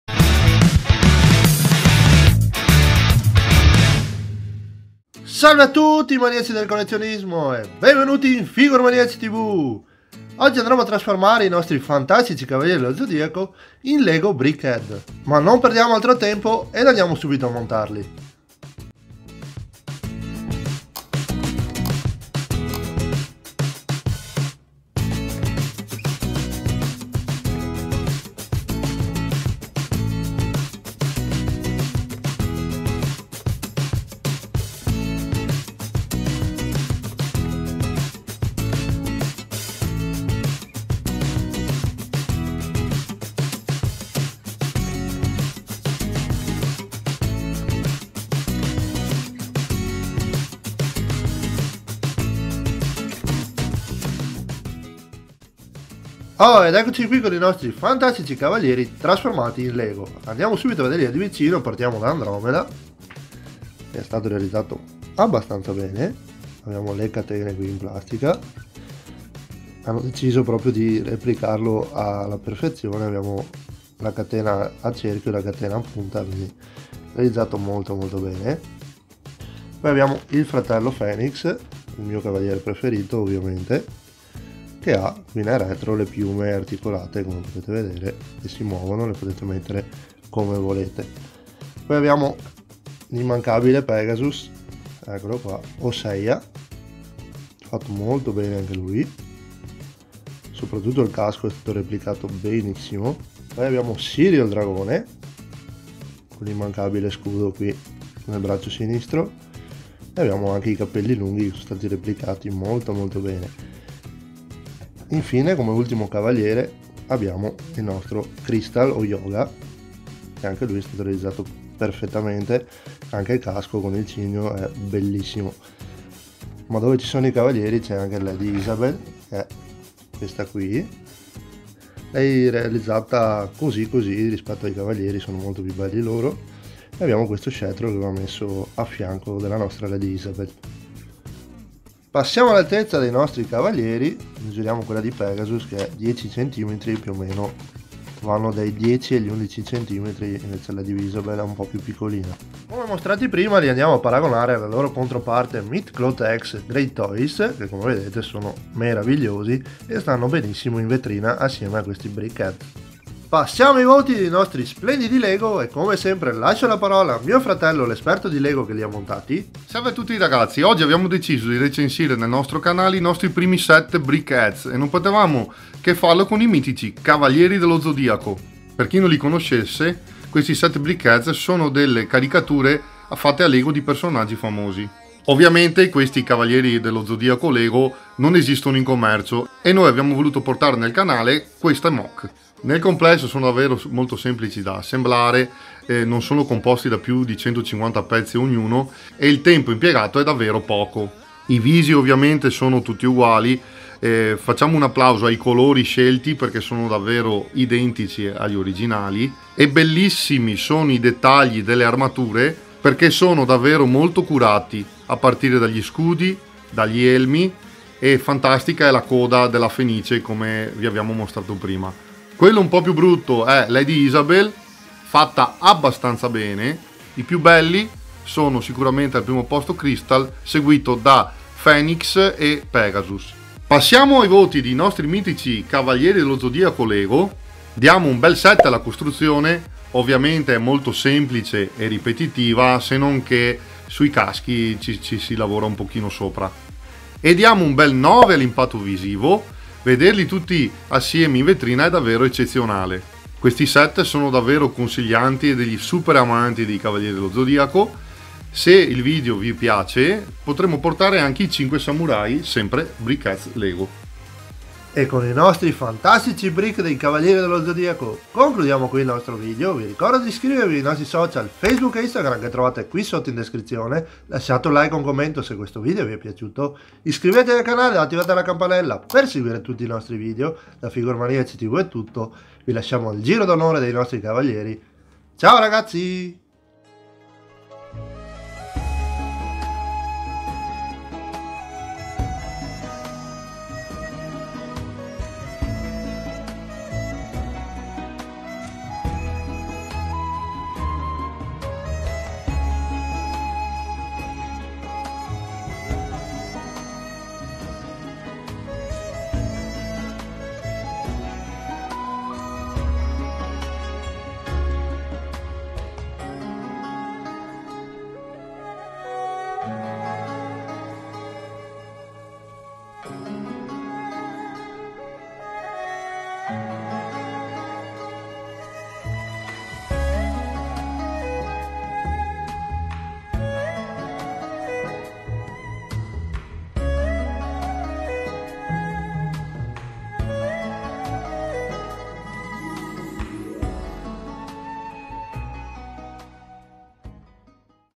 SAINT SEIYA - LEGO MOC BRICKHEADZ - RECENSIONE & MONTAGGIO - ATHENA,SIRYU,SHUN,HYOGA,IKKI E SEIYA!!!
RECENSIONE E MONTAGGIO MOC LEGO BRICKHEADZ ATHENA,SHUN,IKKI,HYOGA,SIRYU E SEIYA!
I CAVALIERI DELLO ZODIACO!

FACEBOOK PER ESSERE SEMPRE AGGIORNATI NEL MONDO DEL CINEMA :)

INSTANGRAM PER TANTE FOTO E RESTARE SEMPRE AGGIORNATI SULLE NUOVE USCITE 😁